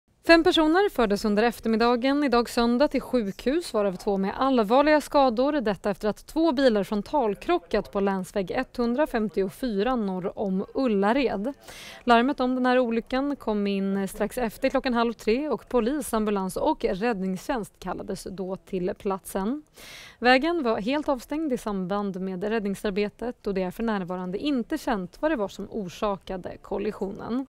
Frontalkrock norr om Ullared – fem personer skadade
Enligt räddningstjänsten har två personer allvarligare skador.